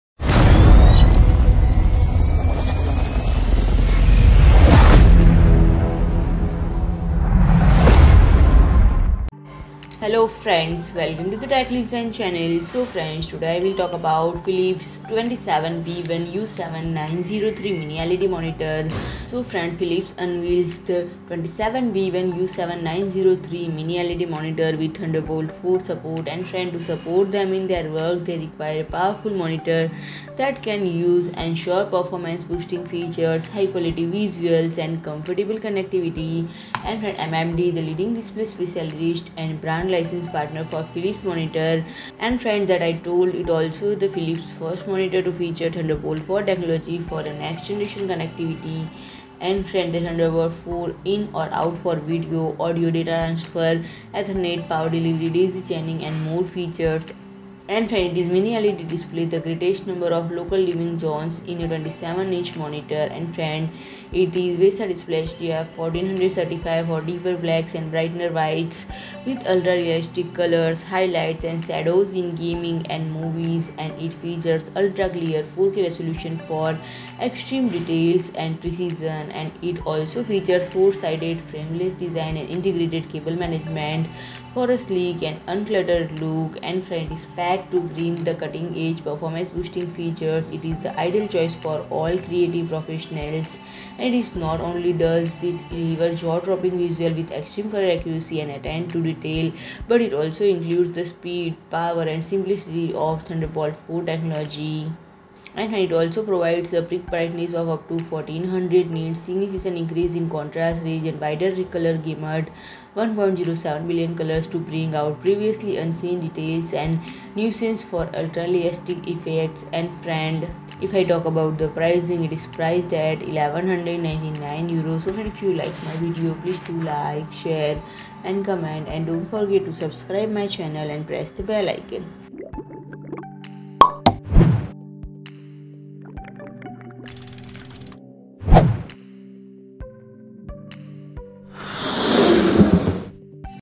Philips 27B1U7903 Mini-LED Monitor Launched With Thunderbolt 4 - Explained All Spec, Features & More
SIMILAR PRODUCT :

Creative professionals and artists work in multifarious ways and look for solutions that meet their individual and specific needs to fully express their creativity. To support them in their work they require a powerful monitor that can ensure performance-boosting features, high-quality visuals, and comfortable connectivity. MMD, the leading display specialist and brand license partner for Philips monitors, today launches the Philips 27B1U7903, a 4K MiniLED Thunderbolt 4 monitor that combines outstanding picture performance with secure one-cable docking, uses sustainable materials in its components and packaging, and comes in a stylish design. Packed with high-performance features to enable professionals to reach new levels of productivity, the Philips 27B1U7903 is also the first Philips monitor to feature Thunderbolt 4 technology for a next-generation connectivity.